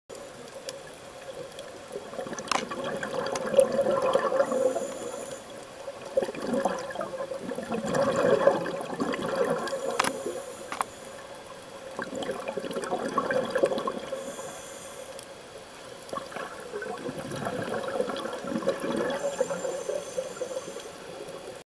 Diving in Cozumel
Morning dive in cozumel - with my wife Denise , son Ryan and bud Neal